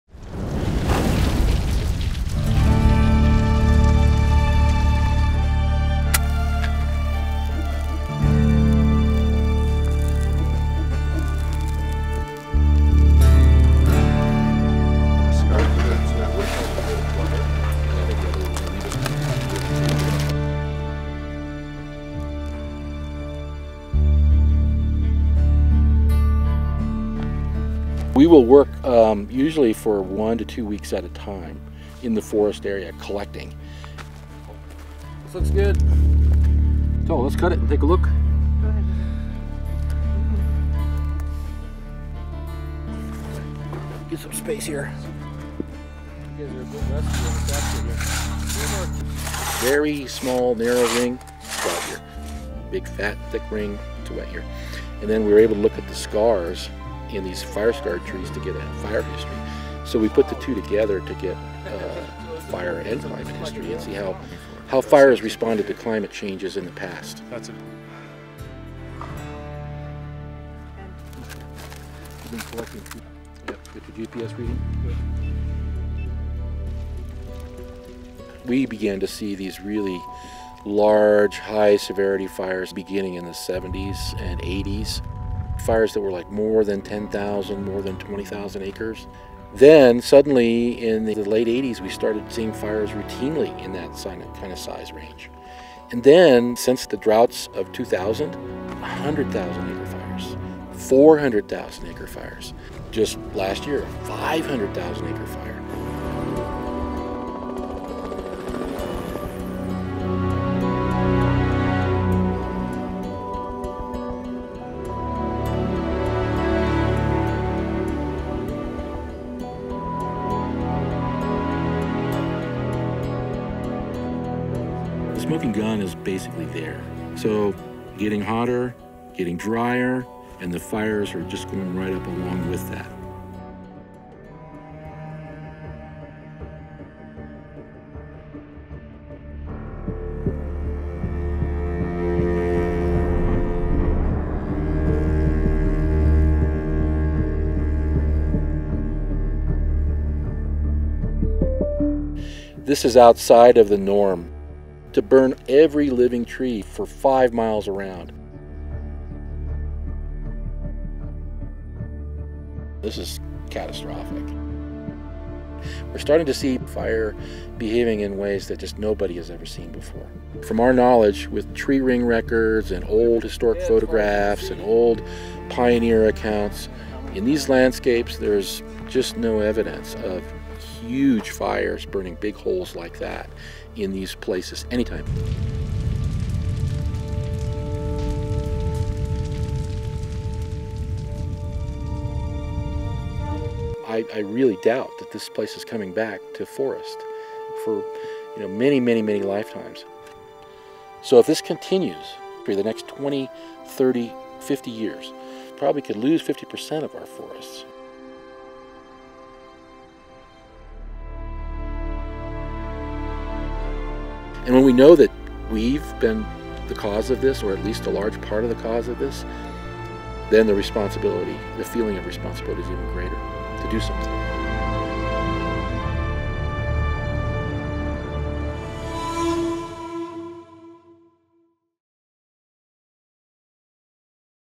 Rings of Fire: Tree Rings Hold The Secret To The Past
Can wildfire season in California get any worse? Former firefighter Tom Swetnam has discovered a way to understand the future of fires by looking into the past. He studies tree rings to learn the secrets of historical droughts and rain.